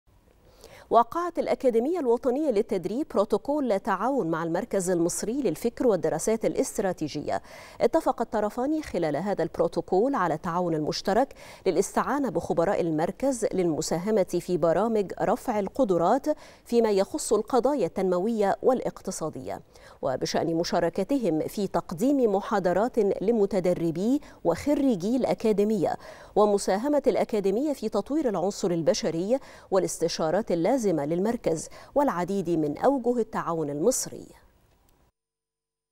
الأكاديمية الوطنية للتدريب توقع بروتوكول تعاون مع المركز المصري للفكر والدراسات الاستراتيجية
نشرة الثالثة | الأكاديمية الوطنية للتدريب توقع بروتوكول تعاون مع المركز المصري للفكر والدراسات الاستراتيجية